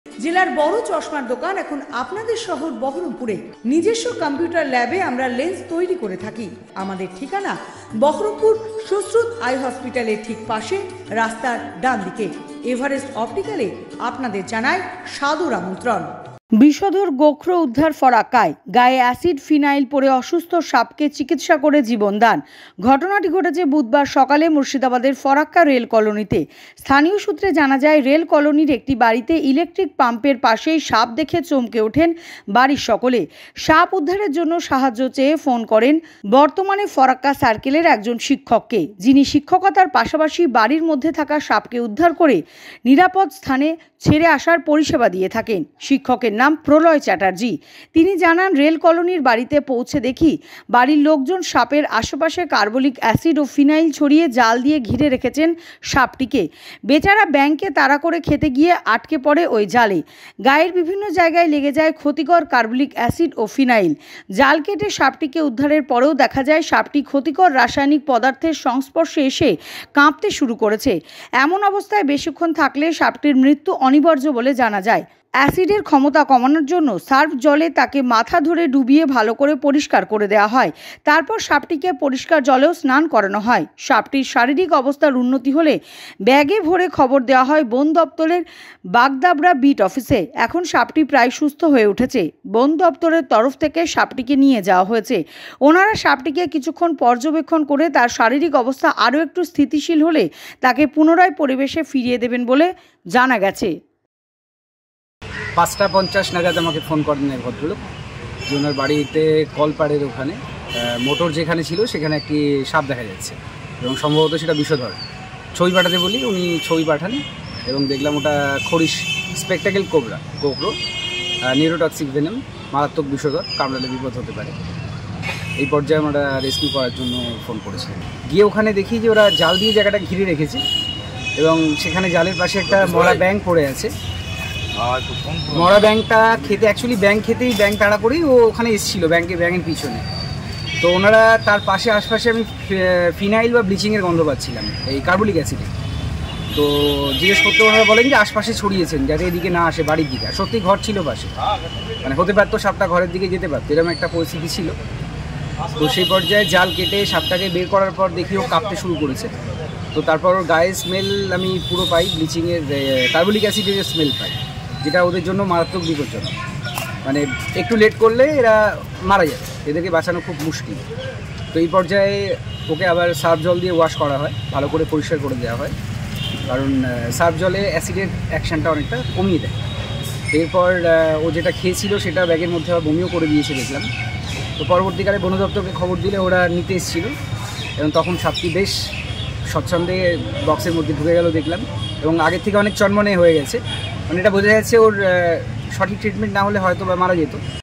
Farakka Sanke: বিষধর গোখরো উদ্ধার ফারাক্কায়
সাপটি কে কিছু ক্ষণ পর্যবেক্ষণ করে তার শারীরিক অবস্থা আরও একটু স্থিতিশীল হলে তাকে পুনরায় পরিবেশে ফিরিয়ে দেবেন বলে জানাযায়।